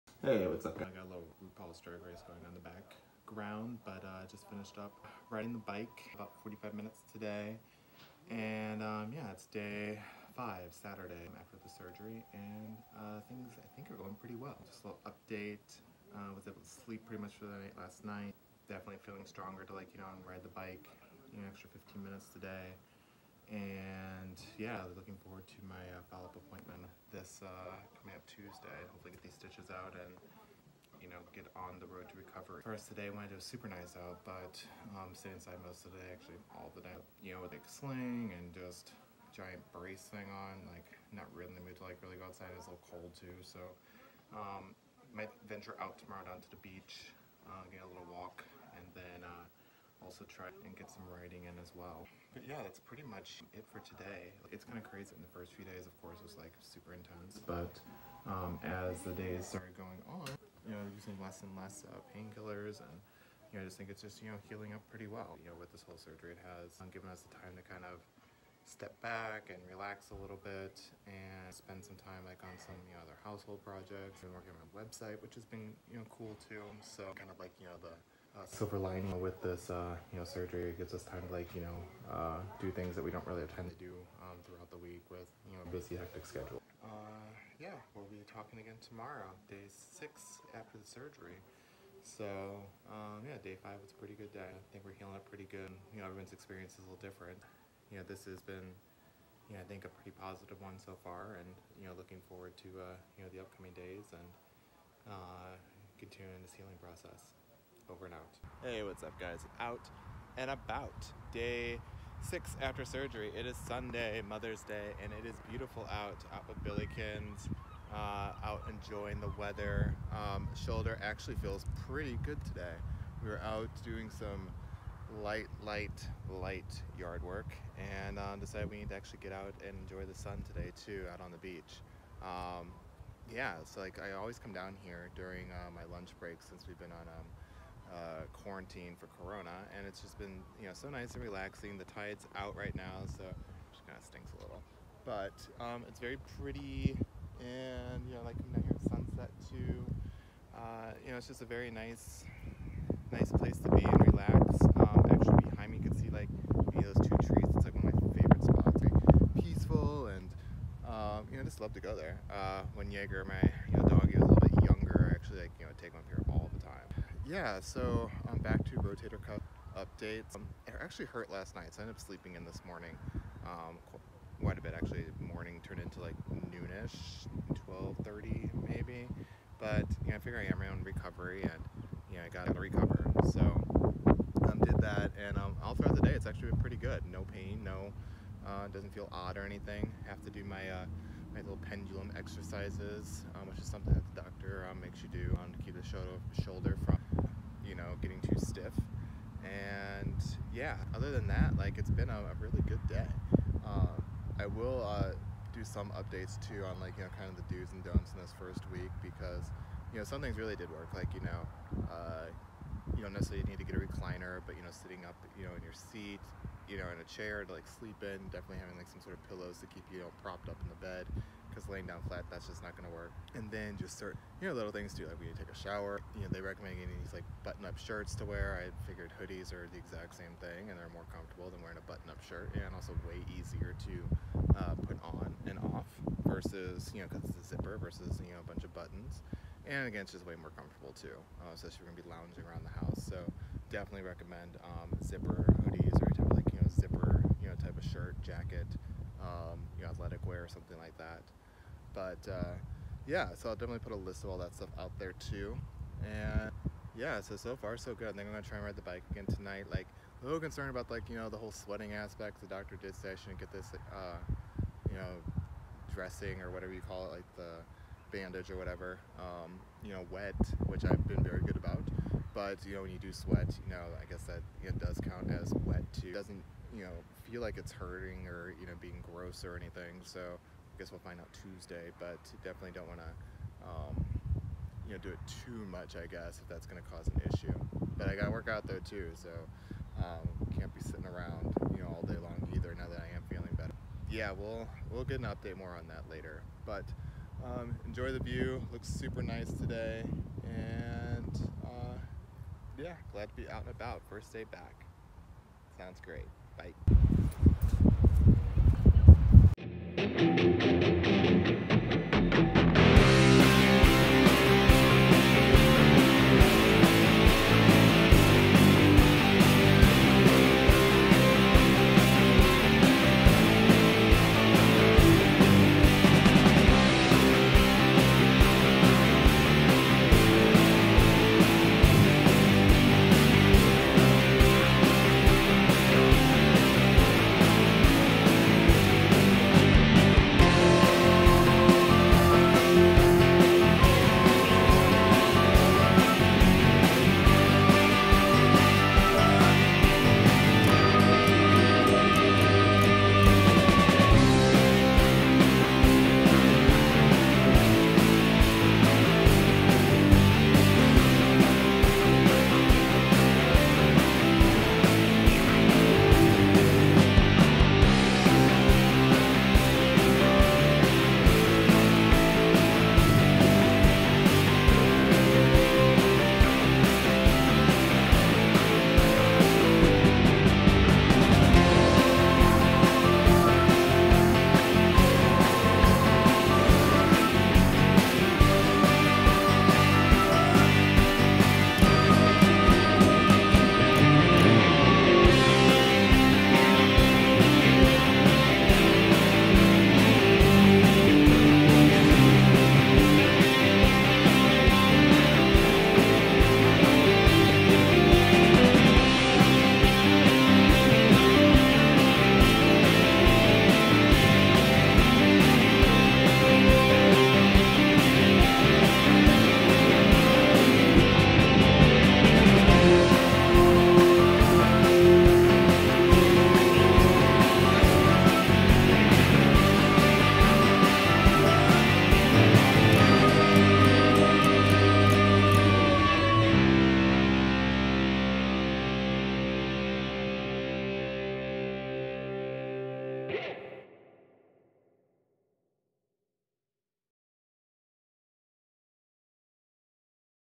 ROTATOR CUFF SURGERY-SURVIVAL-EPISODE 6-The Weekend! + First Week DO's and DONT"S
-Episode 6 PREP & POST OPT of my ROTATOR CUFF SURGERY Survival Journey- The Weekend
FIrst Day out and about!- Finally felt good enough to take a walk down by the beach and get some fresh air :)

Be sure to watch all the way through to see my top rated first week Do's and Don'ts, and for additional inquiries leave a comments and don't forget to like and subscribe!

Insight into my own personal experience of going through a rotator cuff surgery (During the CORONA OUTBREAK) from pre surgery prep to daily and weekly progressions post surgery.  Hopefully providing you with some helpful tips and tricks on what worked/didn't work for me to help you have the most optimal outcome from your rotator cuff surgery.